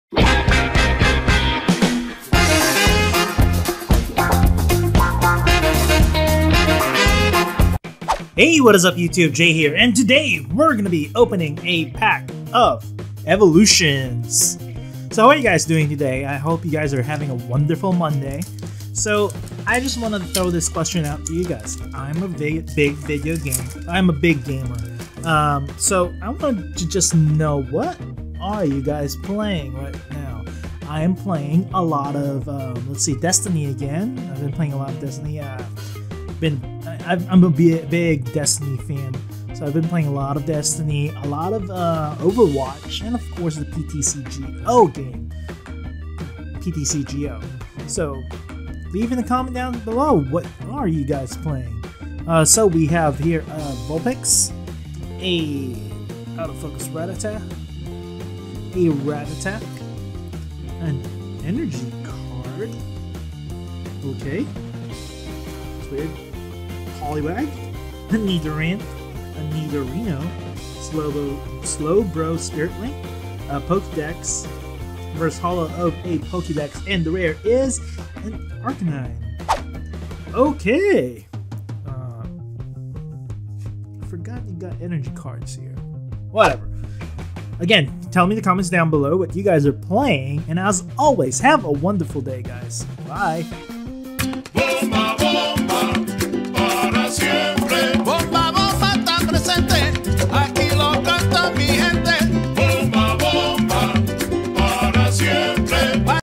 Pokemon Card Daily Opening Day 71
Welcome to another Pokemon Card Daily Opening. My name is Jay and today we are opening a pack of Evolutions. We are now on day 71 of the Pokemon Card Daily Opening series and hopefully we get something good this time. As always have a wonderful day everyone!